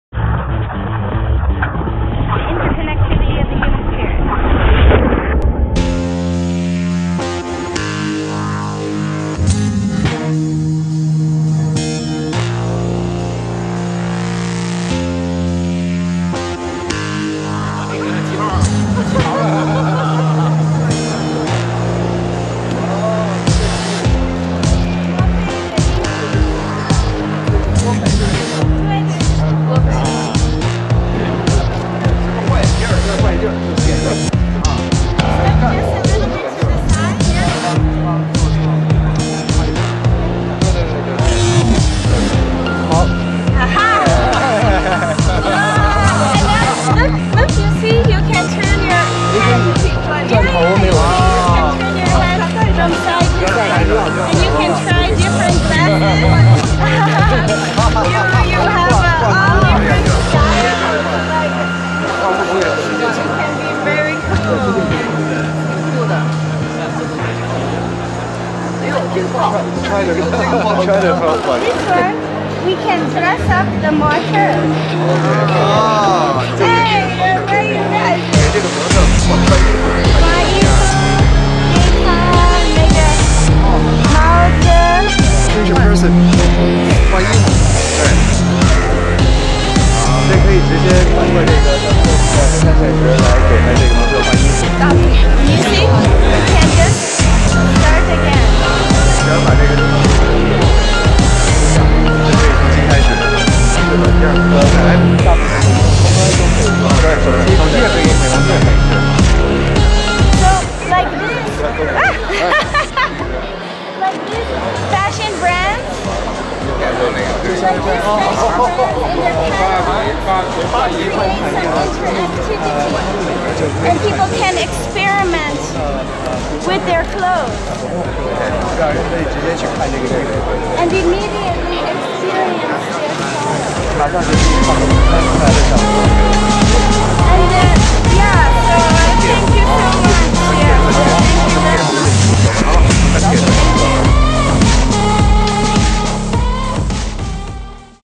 CHIC 2013 visit of Mr. Du Yuzhou at the 360Fashion Network tech zone
360Fashion Network is proud that Mr Du Yuzhou could come visit our booth at the CHIC 2013 in Beijing. We could only show Mr Du three of our technologies, we think he liked our technology. Mr Du is a great chairman and we hope that he enjoyed his visit.